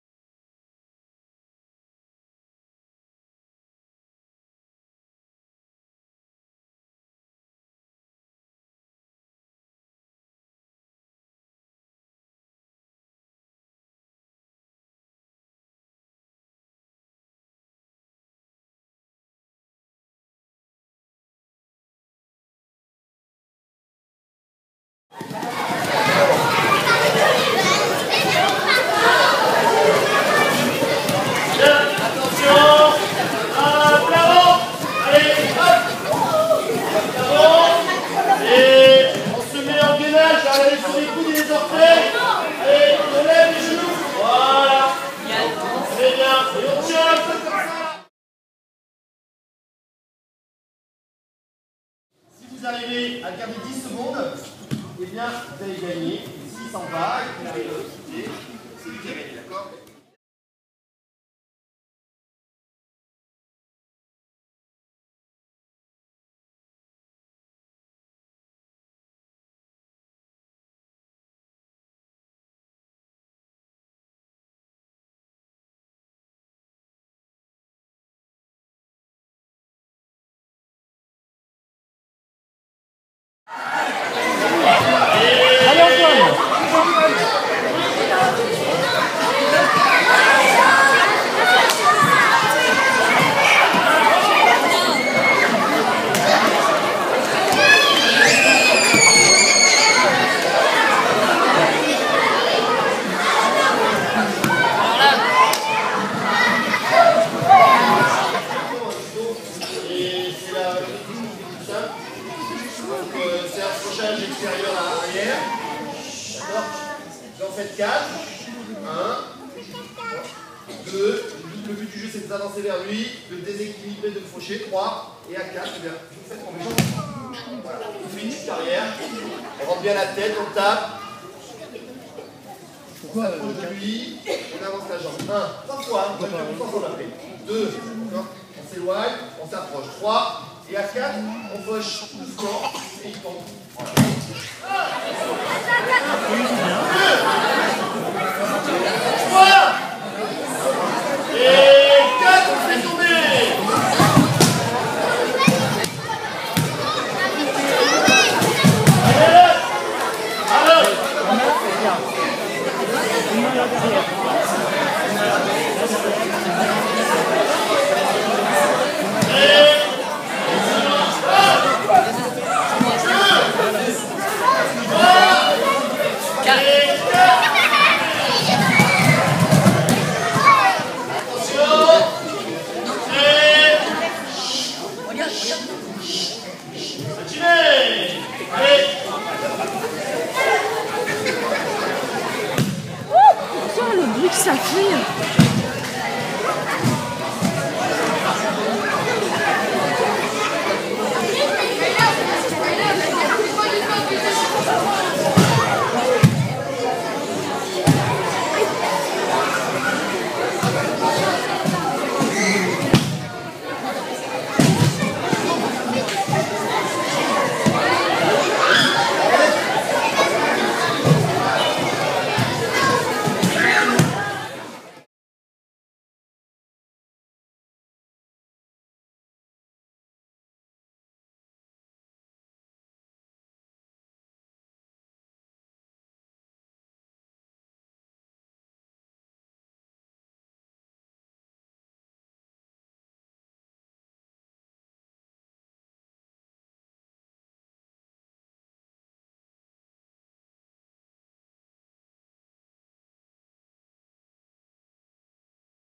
Téléthon 2016
Téléthon édition 2016. Entraînement portes ouvertes à 2€.
Nous avons récolté 500 € merci pour votre générosité. Rendez vous l'année prochaine.